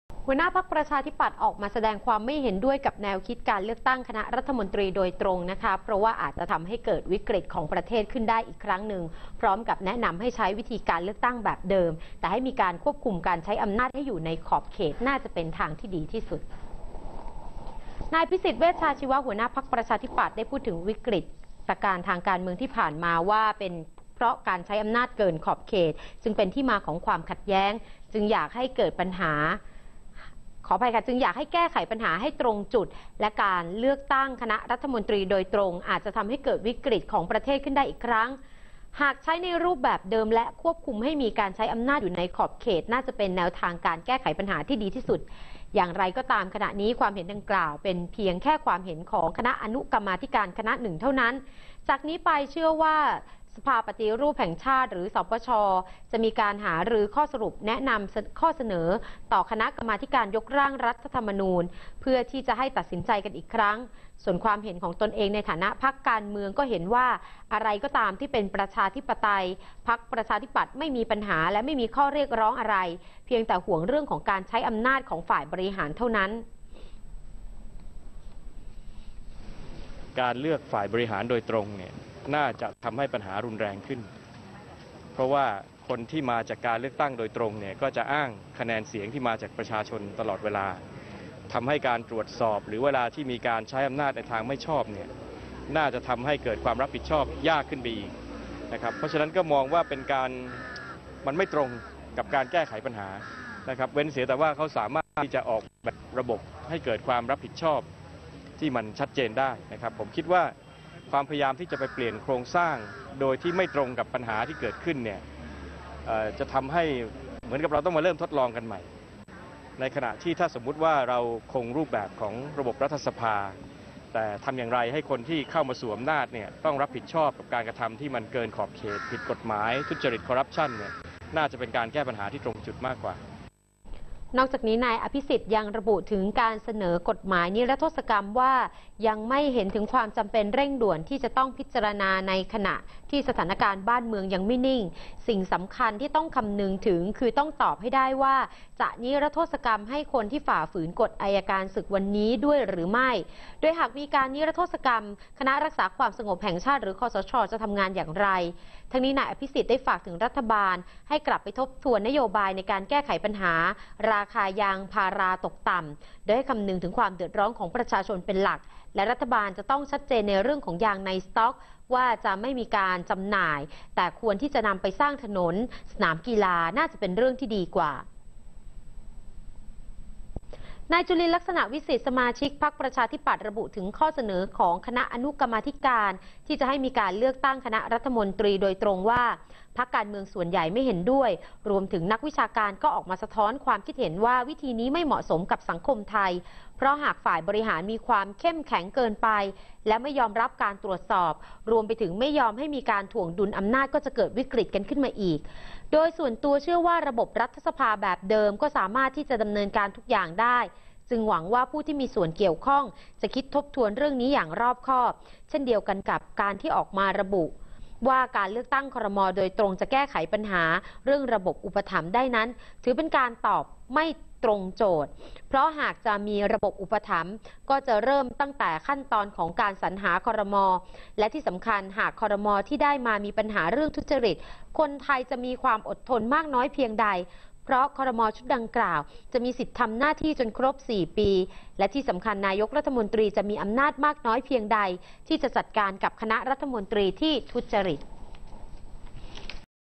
"อภิสิทธิ์" ไม่เห็นด้วยเลือกตั้งครม.โดยตรง
2014/12/10 "อภิสิทธิ์" ไม่เห็นด้วยเลือกตั้งครม.โดยตรง